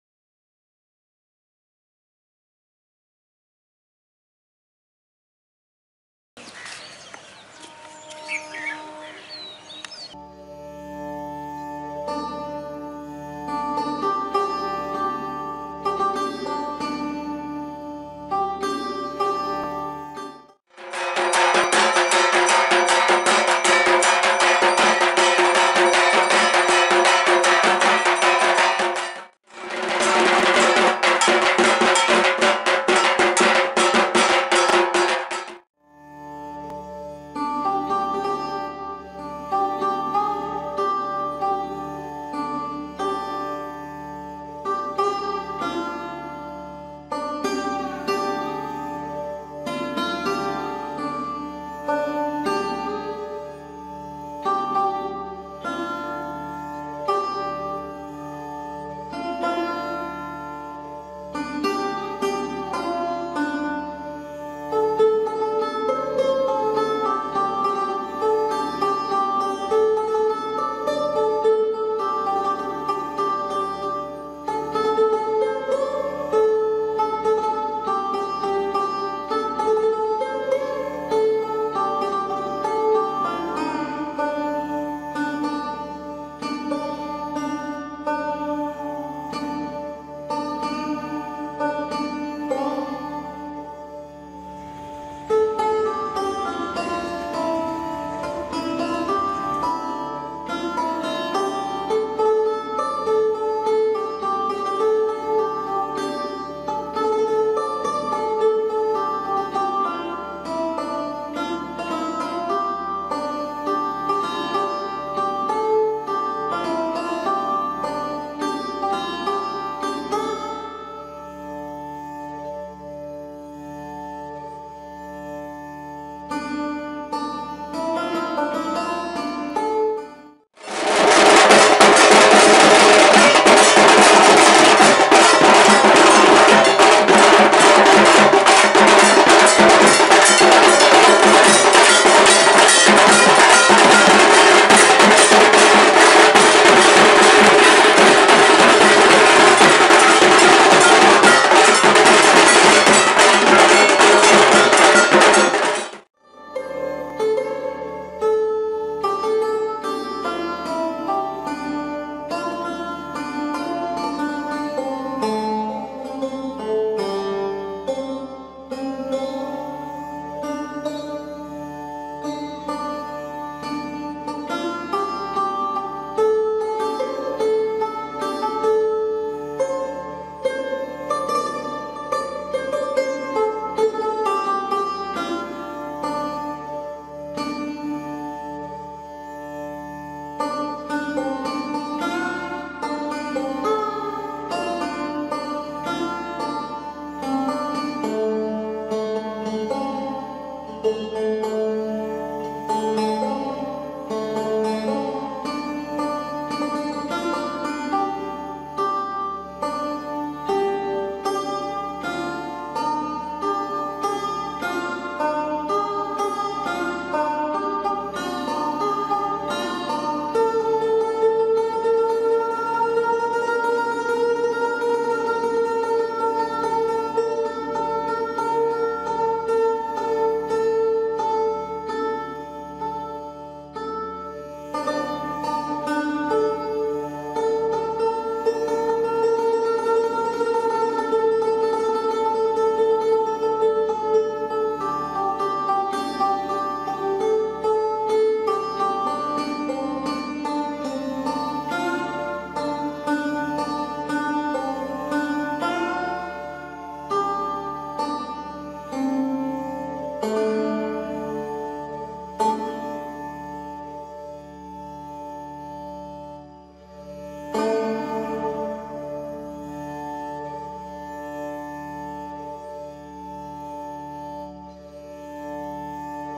Durga Puja 2016 Mahalaya
Mahalaya is the Beginning of Durga Puja 2016 - The Greatest Festival of India. Happy Durga Puja 2016